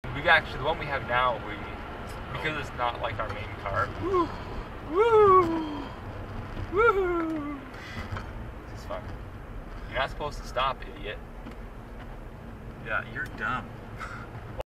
Idiot stops on I15 in the middle of a snowstorm, almost gets rear ended
Seems as good a place as any to stop, I went slower after this